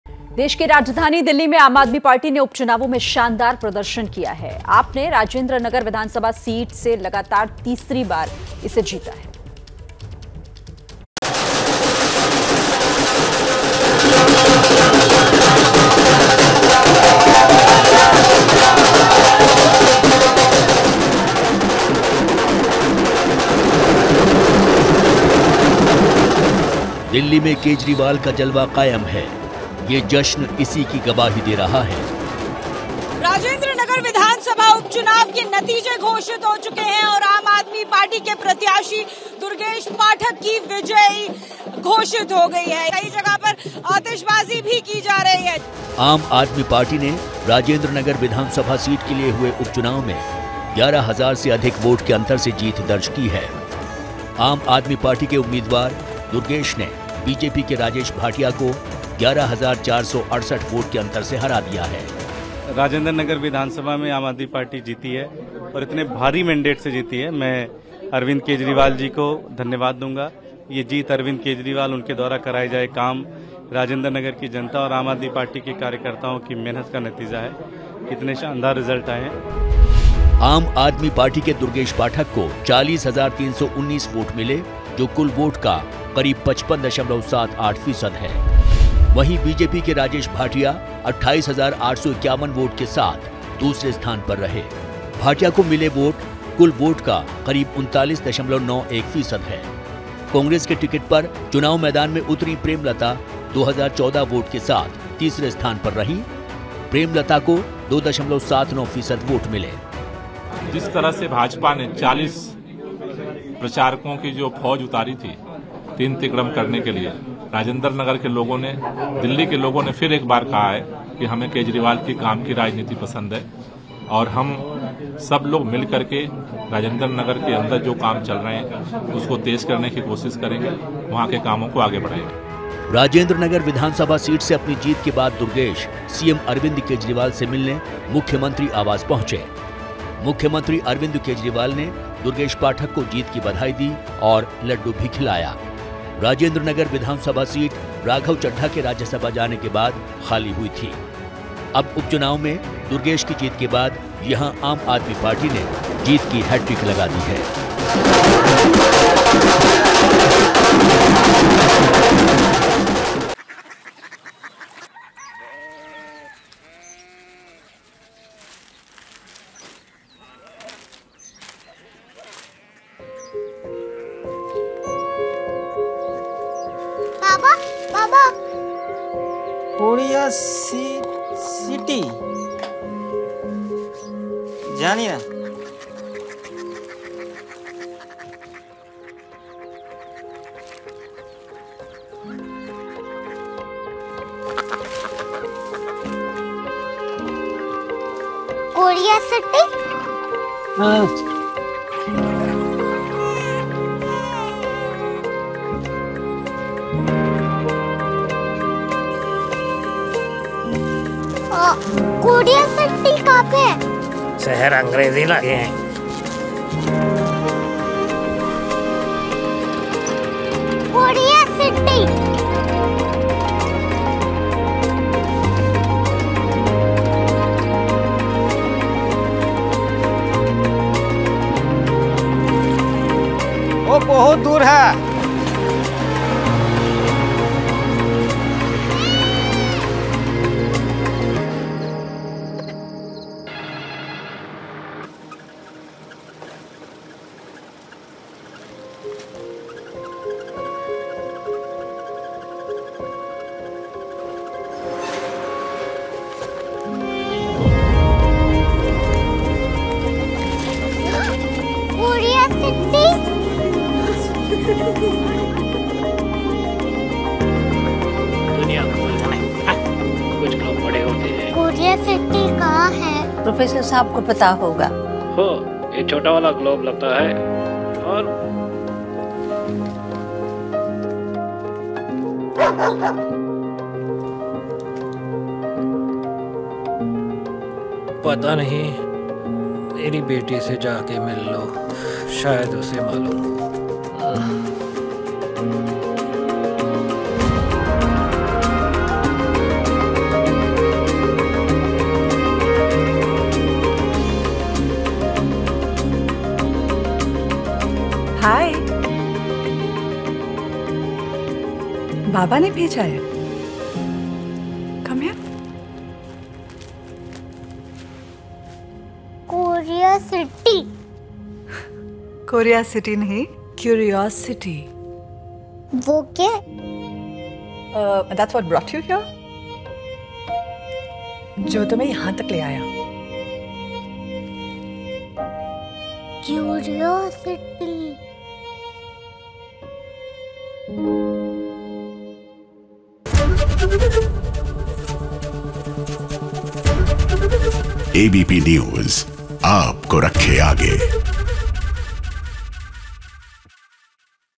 Delhi में आम आदमी पार्टी(AAP) ने उपचुनावों में किया शानदार प्रदर्शन
देश की राजधानी दिल्ली  में आम आदमी पार्टी ने उपचुनावों में शानदार प्रदर्शन किया है. आप ने राजेंद्र नगर विधानसभा सीट लगातार तीसरी बार जीत ली है